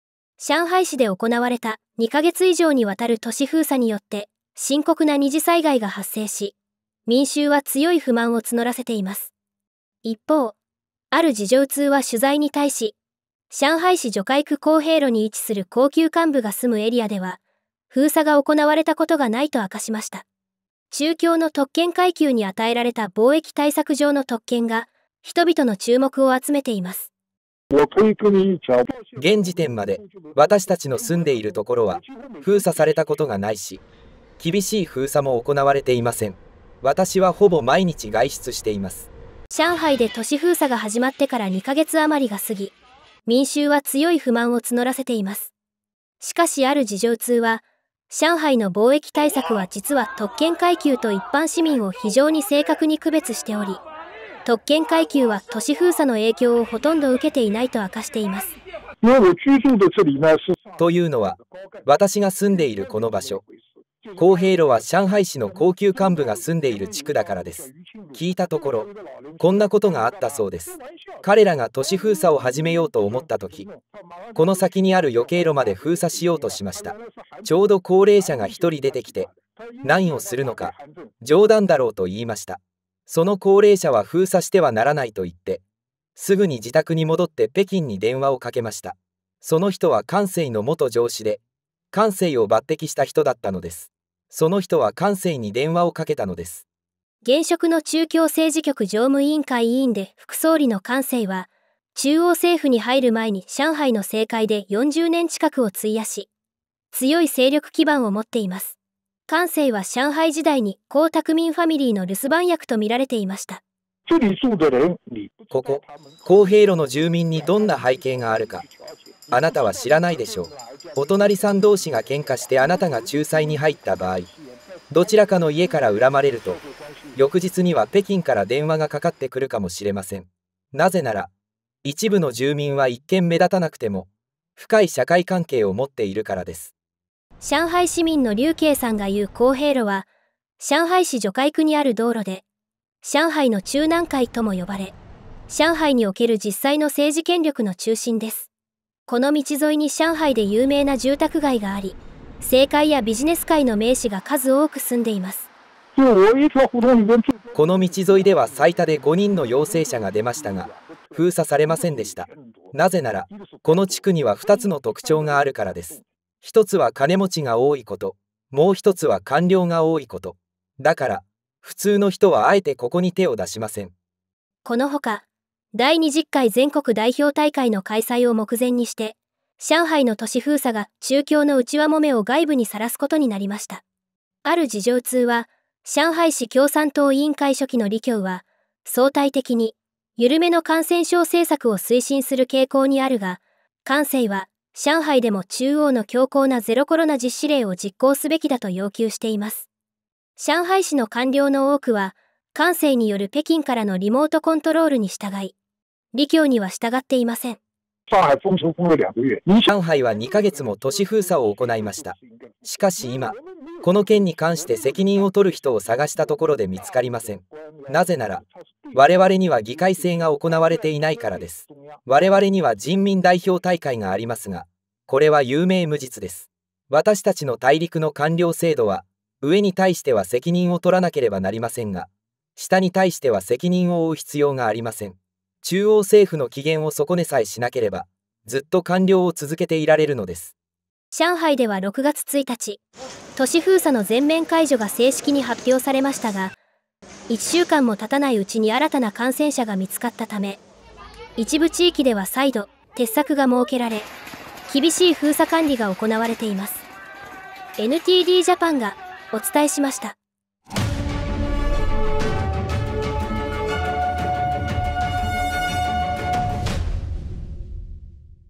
上海市内に都市封鎖免除区域 「特権階級が住んでいるから」
上海市で行われた２か月以上にわたる都市封鎖によって深刻な二次災害が発生し、民衆は強い不満を募らせています。一方、ある事情通は取材に対し、上海市徐匯区康平路に位置する高級幹部が住むエリアでは封鎖が行われたことがないと明かしました。中共の特権階級に与えられた防疫対策上の特権が人々の注目を集めています。　　＊＊＊
預金種目：普通預金
口座番号：2158157
口座名義：ﾄｸﾋ)ｴﾎﾟｯｸﾒﾃﾞｨｱ